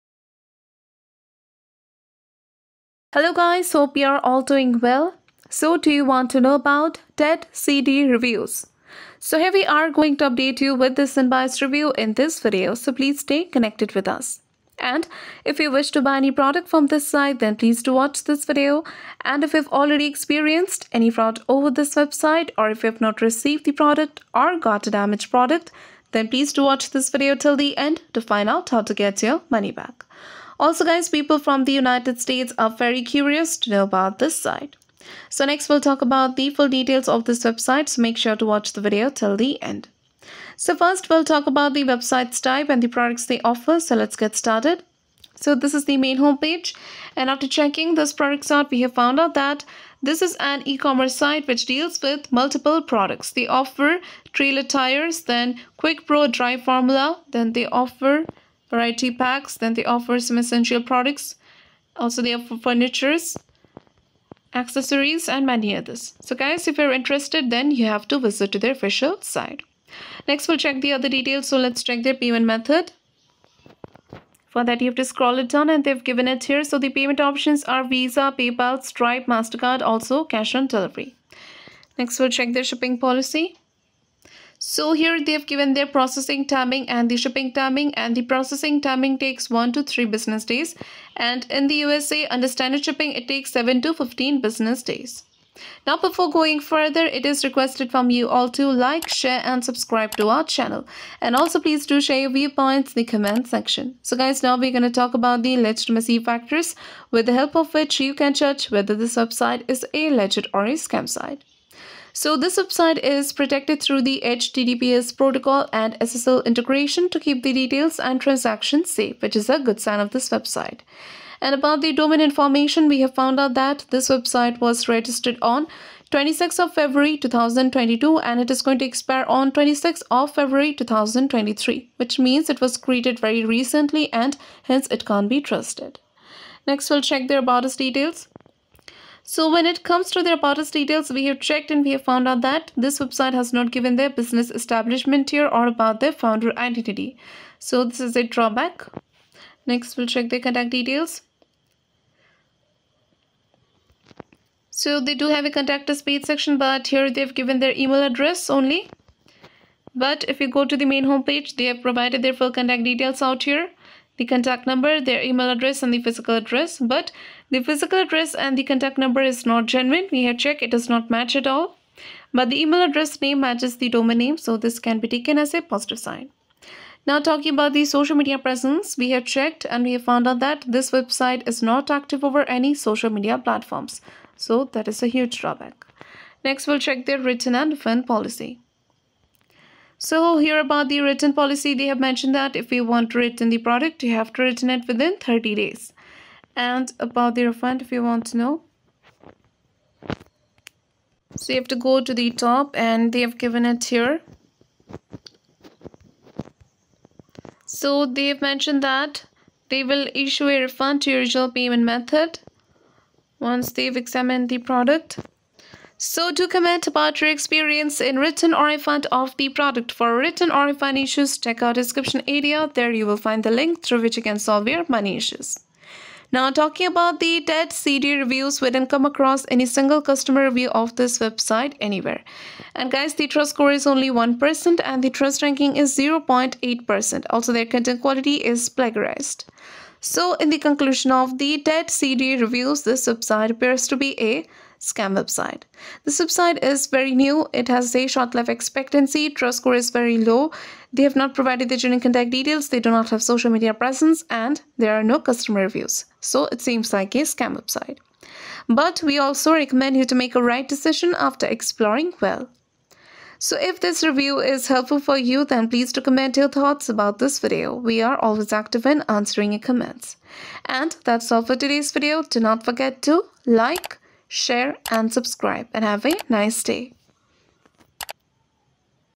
Tedcd Reviews (April 2022) - Is This A Safe Hub For Online Shopping? Find Out! | Scam Inspecter
Do you want to know about Tedcd Reviews?  Watch this video till end because in this video you will get to know about the legitimacy of the site.

Address: 400 W Main St #2601, Durham, NC 27701, USA

Scam Inspecter